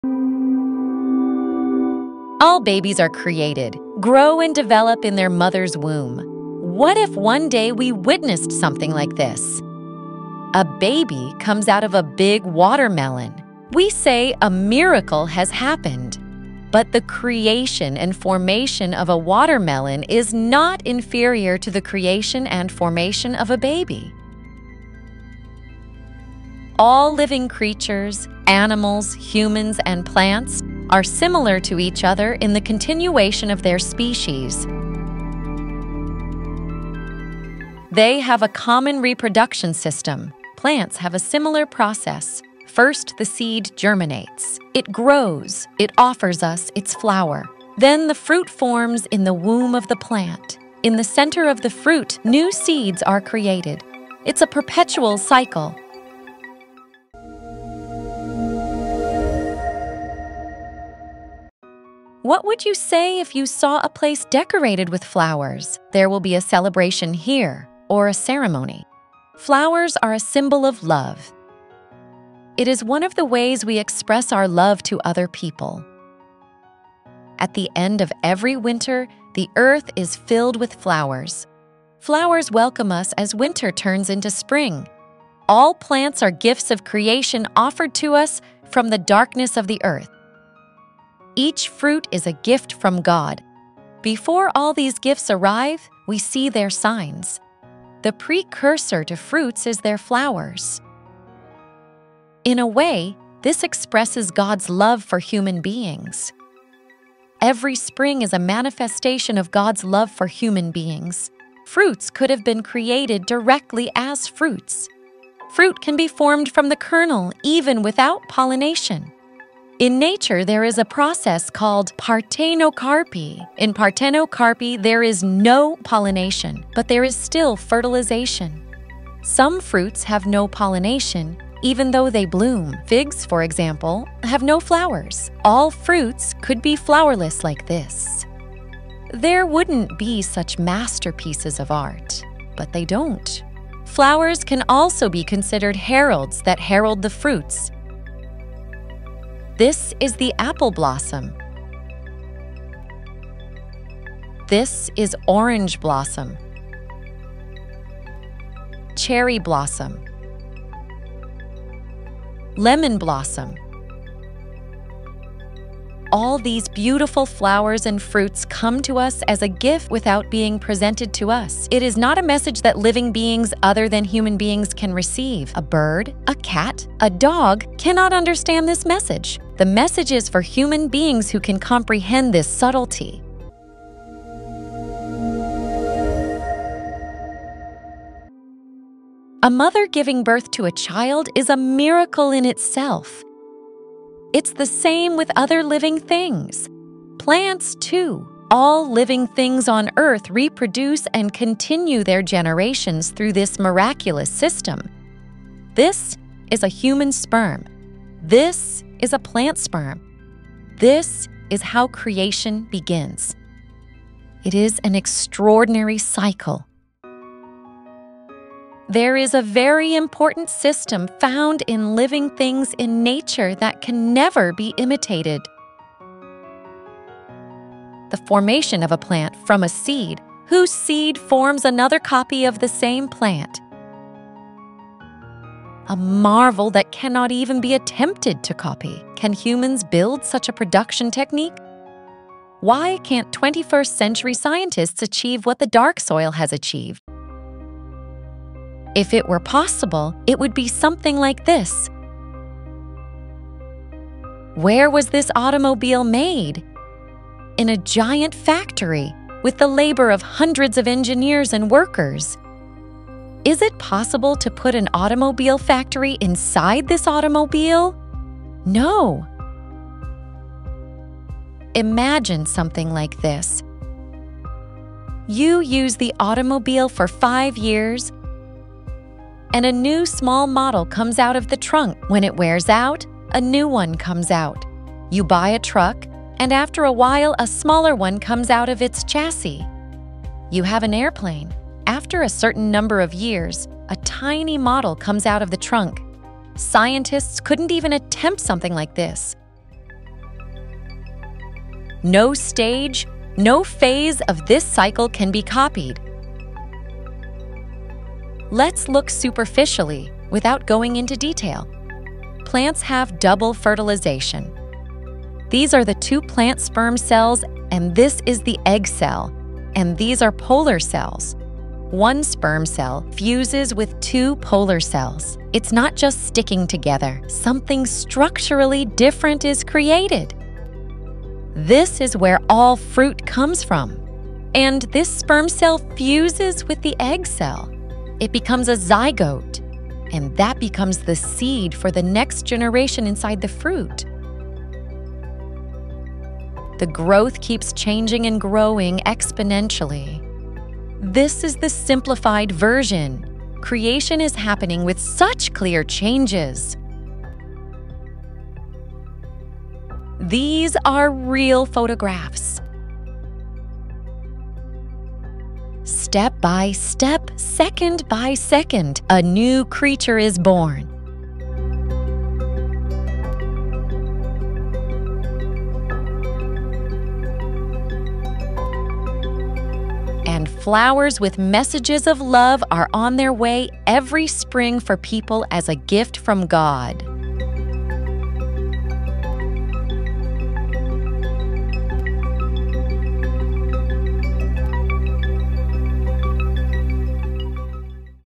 Does God send flowers?
ONTOLOGICAL QUESTIONS
"What does creation mean?
A mother giving birth to a child is a miracle in itself.
It's the same for other creatures.
So are plants.
All living things on earth reproduce and continue their generations with this miraculous system.
This is human sperm. This is a plant sperm.
This is how creation begins.
The formation of a plant from a seed, and then a seed emerging from it re-forms the same plant...
An extraordinary revolution.
A marvel that cannot even be attempted to be copied.
Could humans establish such a production technique?
Why can't 21st century scientists achieve what the pitch black earth achieved?
If it were possible it would be something like this:..."

Text and compilation: Veysel Ayhan
YOUTUBE IMAGES AND INFORMATION SOURCES
Dall-e,
ChatGBT
Fertilization of Plants Live-Cell Imaging Reveals the Dynamics of Two Sperm Cells during Double Fertilization in Arabidopsis thaliana
Fertilization in flowering plants: an odyssey of sperm cell delivery
Central cell Sperm cells Ovule Vegetative nucleus Synergid cells pollen tube Egg cell Current Opinion in Plant Biology Schematic representation of sperm cell behavior during double fertilization in Arabidopsis